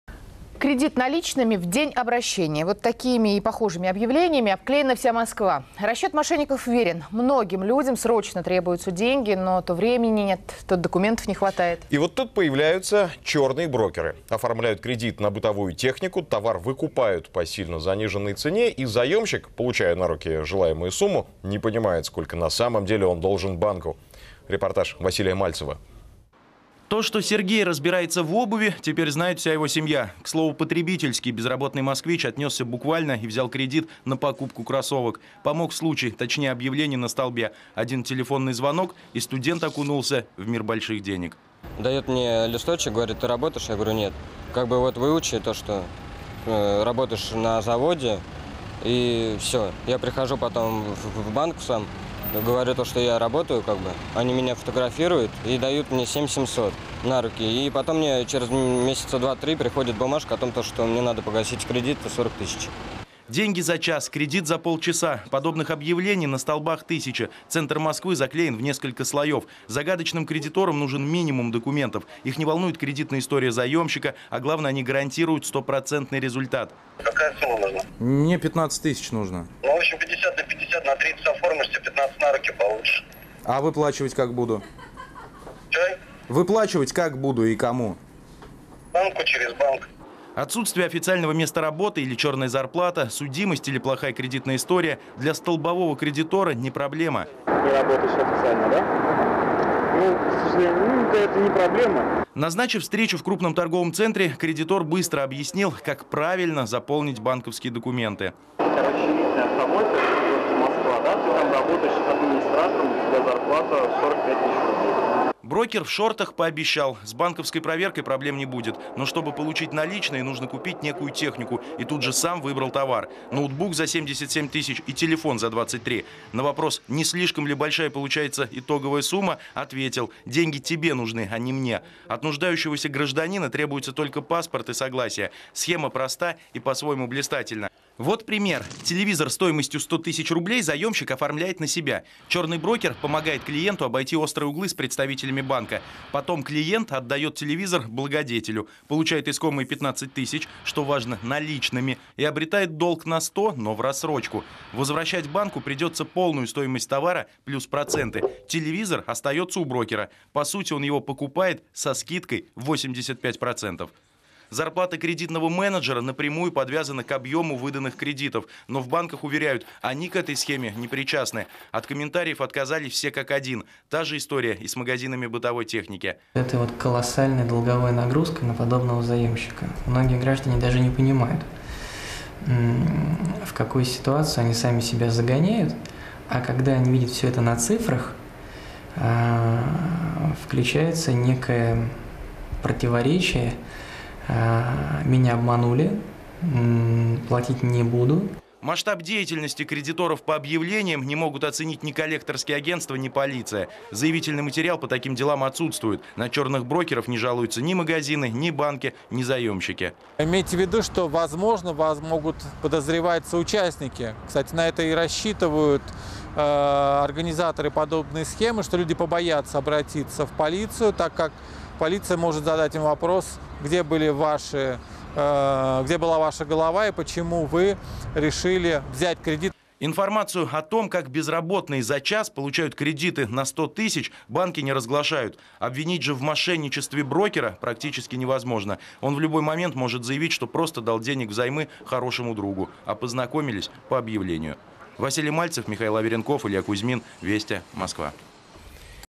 vesti-kredit.avi
фрагмент выпуска новостей о криминальных кредитах.
все остановки уляпаны объявлениями - деньги в кредит без справок и т.п.